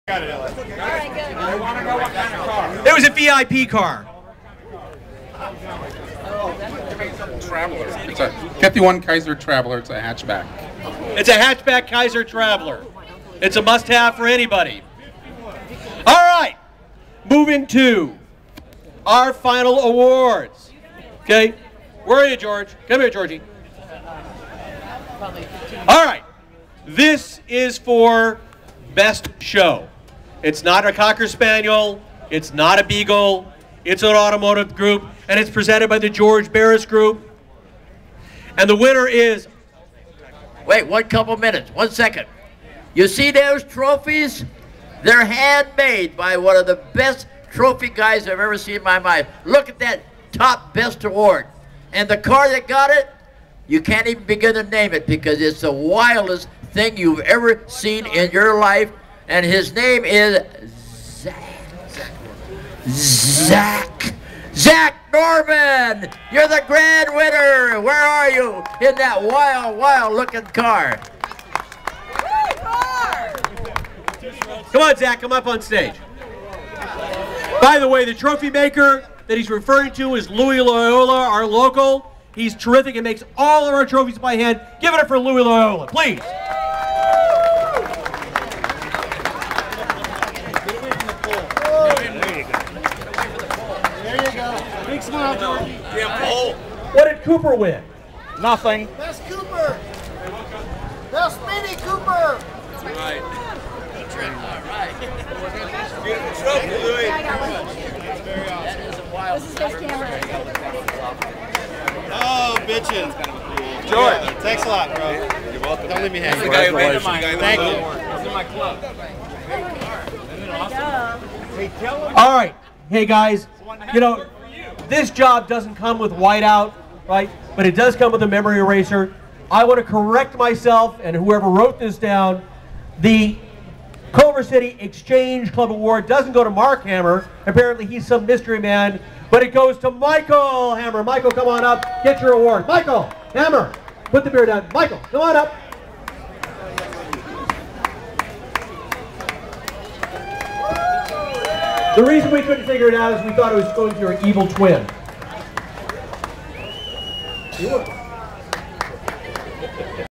George Barris Awards Time,at the car Show 5-7-2011,in Culver City,CA. Back to the 50's Car Show.
Donald Randell's Cars and Highlights,at George Barris Car Show 5-7-2011 in Culver City,CA. (Awards Time).